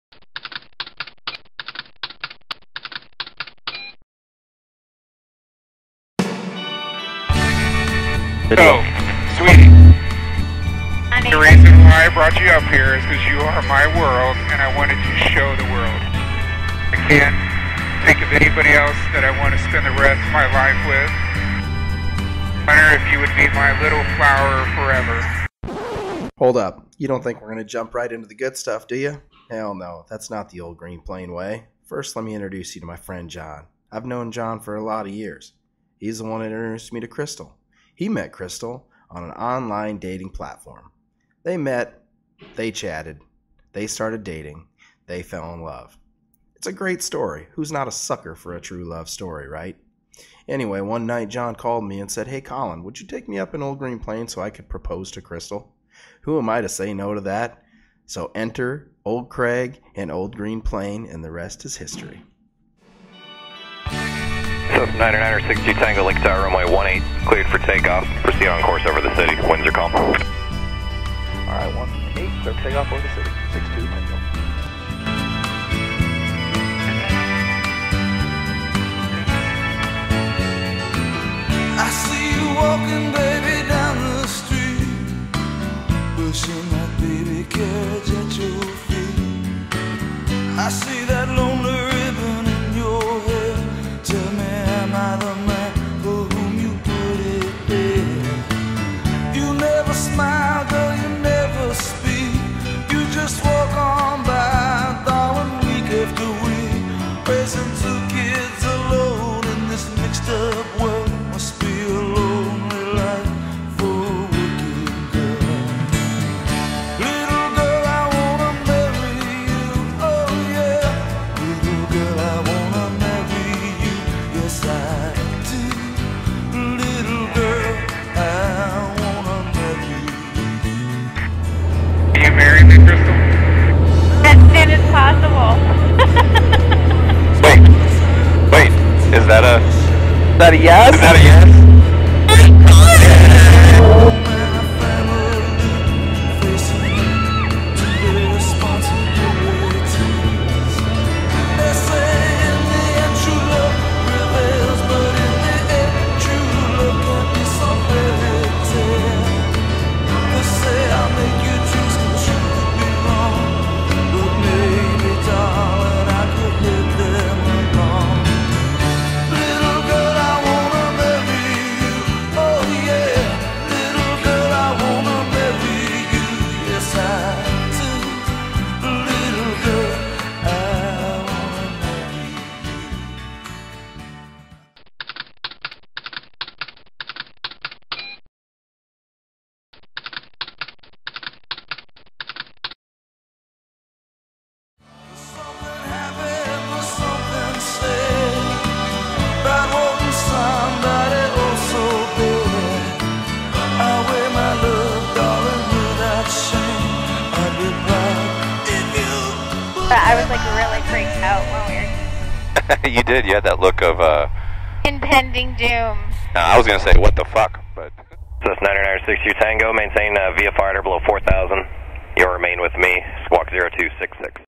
Old Green Plane_ Marriage Proposal
In this episode, a friend asks the OGP Crew if he could propose to his girlfriend in the friendly skies... Who are we to say no to that!?